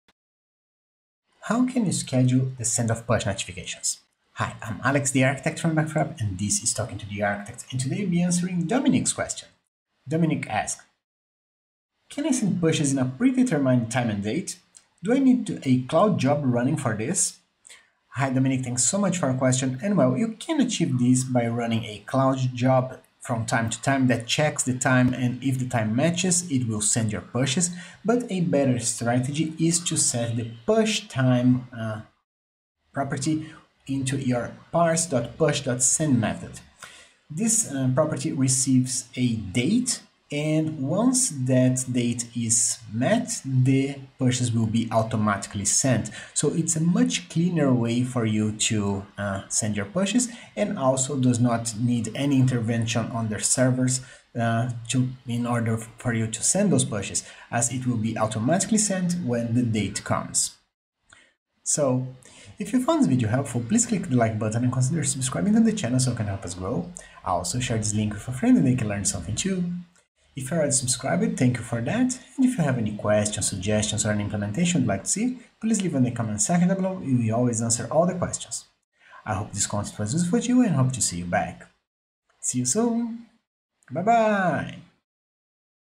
Scheduling Pushes
Today in Talking to the Architect, we will see how to schedule Push Notification sending.